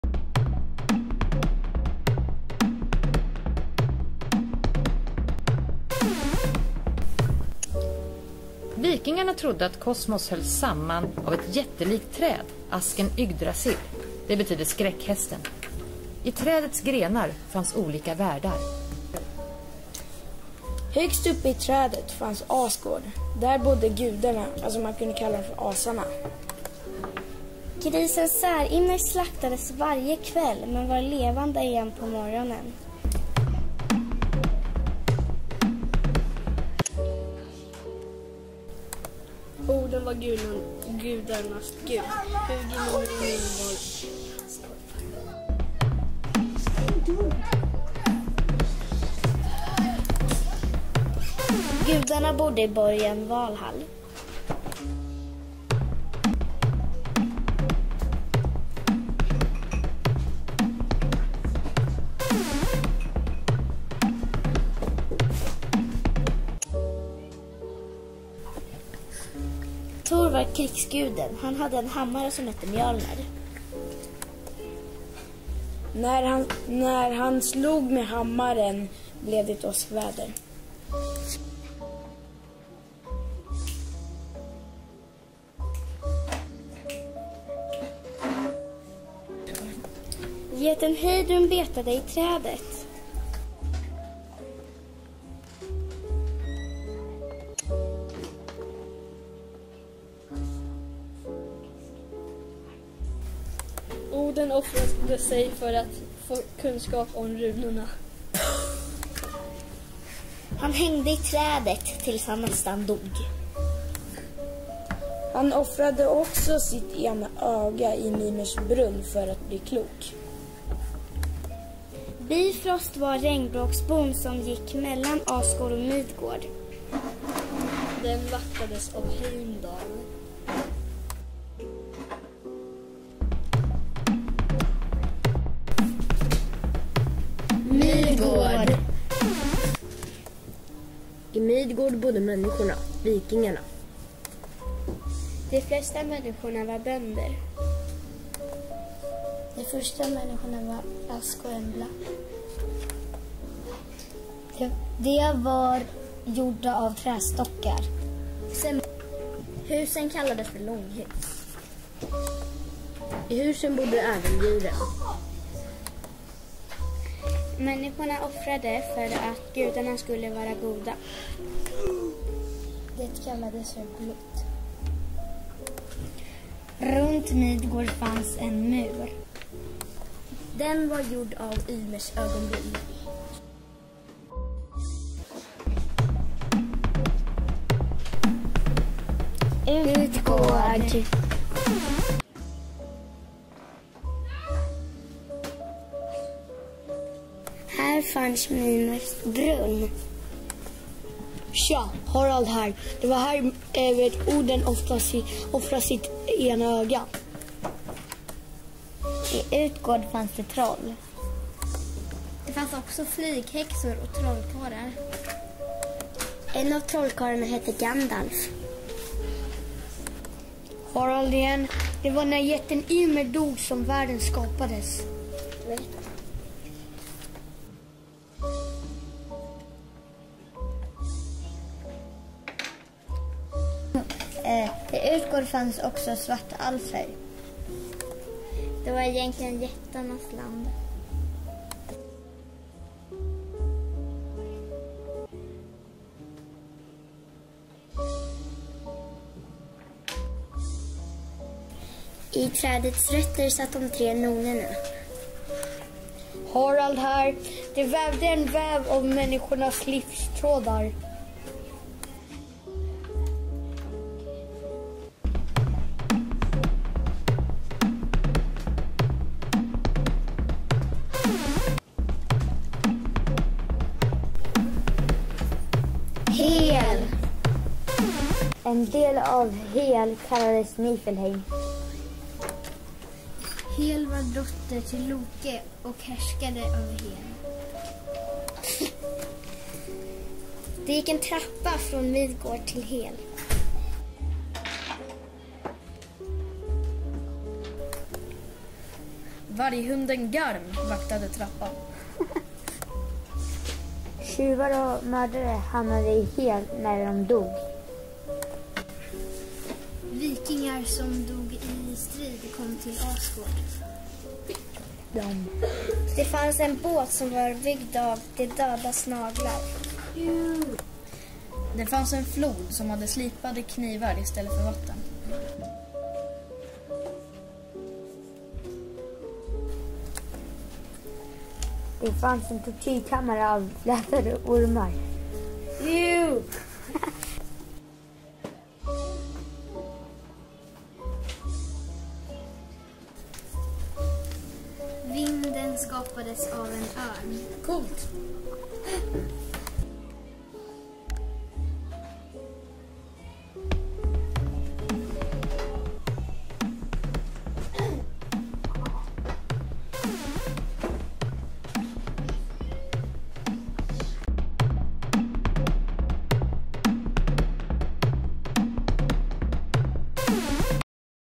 Yggdrasil klar
En film om vikingarnas världsbild av klass 4c2 Björknässkolan.